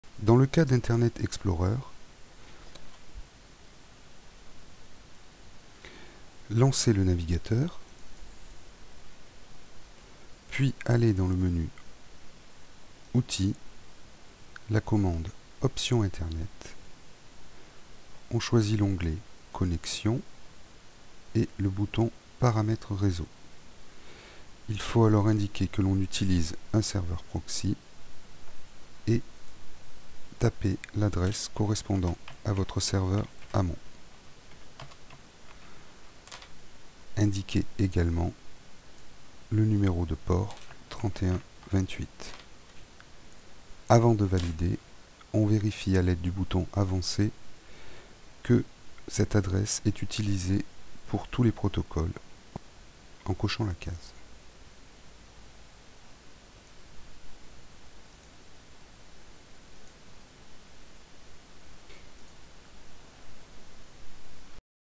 [TUTO-03-IE] Comment accéder à internet avec un poste non-ENR (Internet Explorer)
Serveur EOLE dans le cadre des ENR EC Bretagne